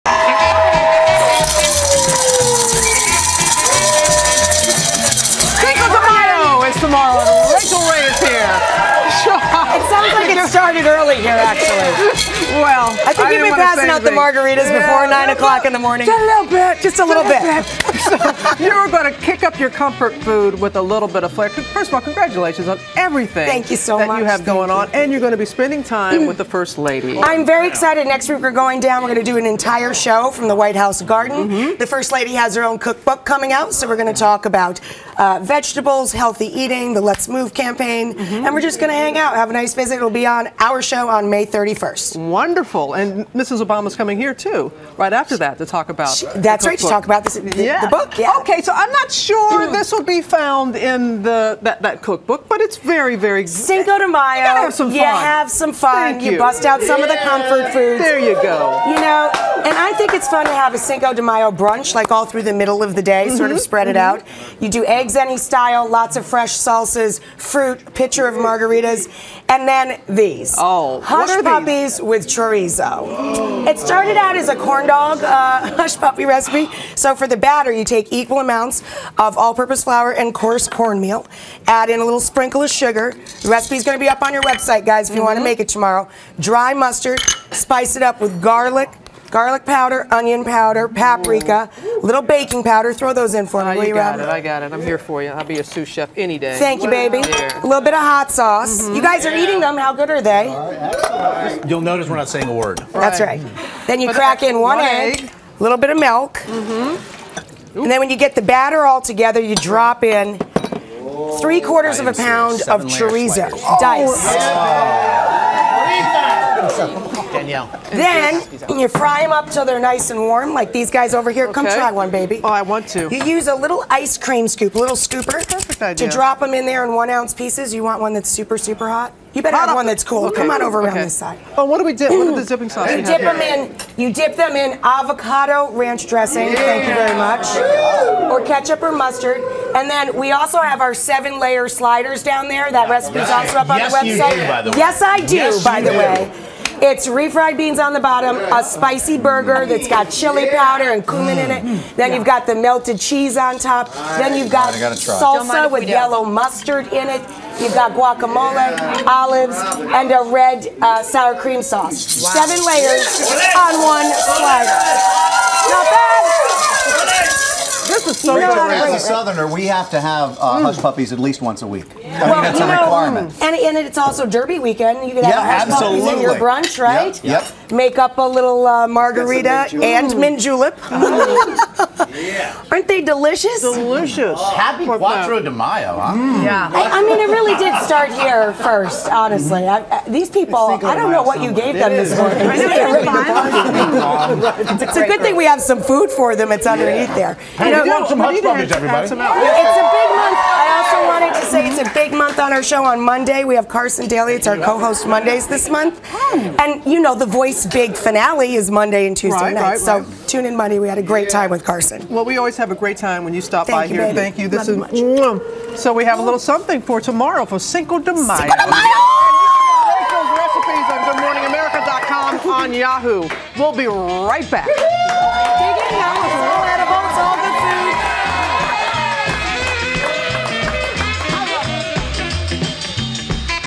Cinco de Mayo Recipes: Rachael Ray's 7-Layer Sliders and Hot Dog Hush Puppies - Party Appetizers!
TV chef prepares dishes perfect for a comfortable and tasty Cinco de Mayo.